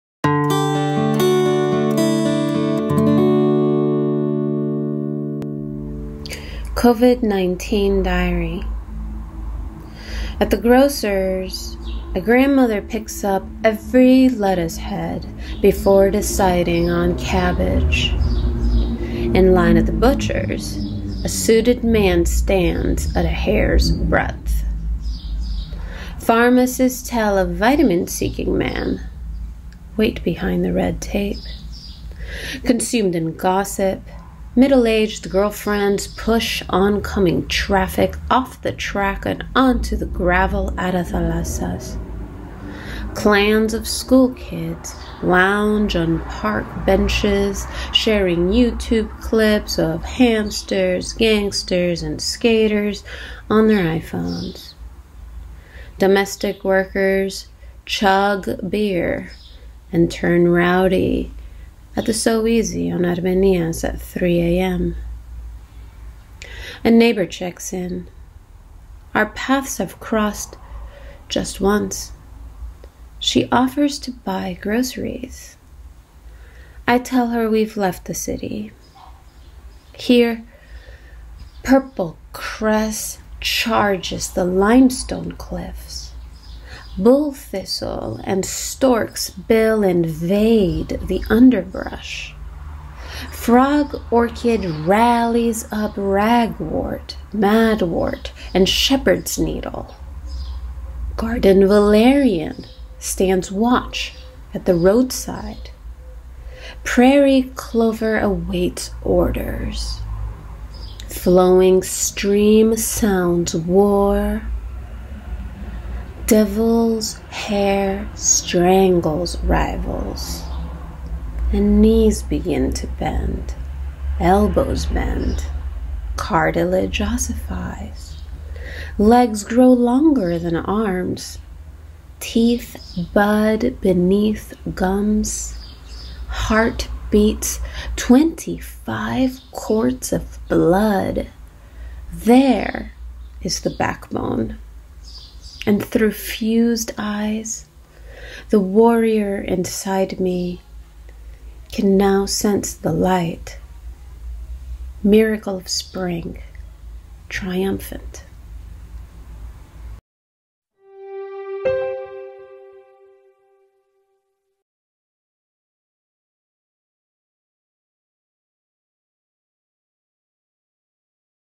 WohnzimmerGedichte: "Covid-19 Diaries", Marilena Zackheos
Following the translations workshop "Crossing" with poets from Germany and Cyprus in the first quarter of 2019, we invited them approximately a year later to share one of their poems with us and to take part in our virtual WohnzimmerGedichte (living room poems) readings.

Some of the poems were written during or right after the translation workshop; other poems were written as a reaction to or as an observation of the unprecedented situation we find ourselves in globally, because of the COVID-19 pandemic.

Participating poets are: Senem Gökel, Lily Michaelides, Giorgos Moleskis, Nora Nadjarian, Achim Wagner, Marilena Zackheos